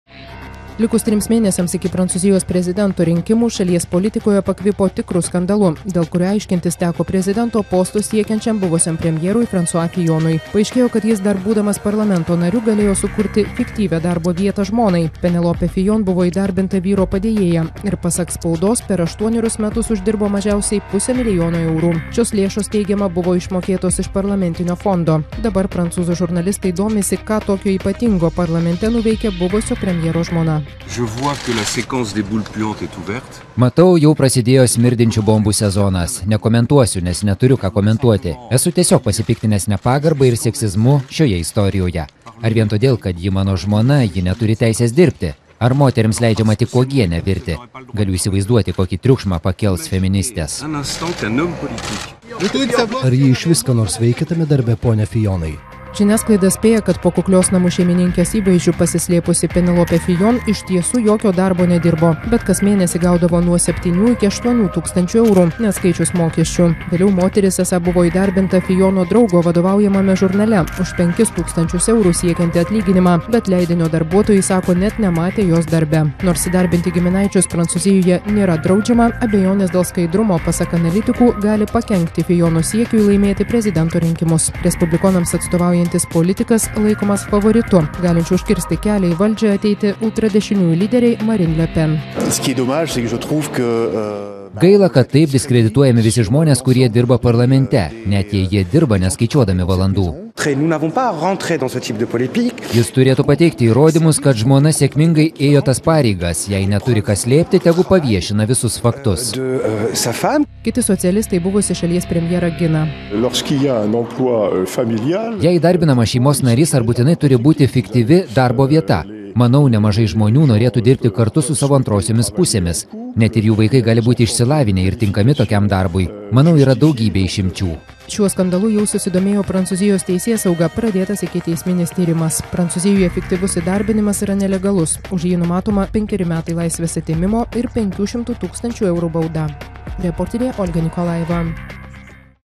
Lietuviams gerai pažįstamas scenarijus sukėlė tikrą audrą Prancūzijoje
Šalies spauda atskleidė, kad buvęs premjeras Fransua Fijonas, dar būdamas eiliniu parlamentaru, parlamente įdarbino ir žmoną. Penelopė Fijon aštuonerius metus dirbo vyro padėjėja ir per šį laiką uždirbo pusę milijono eurų. Pasak spaudos, pinigai buvo neteisėtai išmokėti iš parlamentinio fondo. Prezidento rinkimų favoritu laikomas politikas visus kaltinimus atmeta.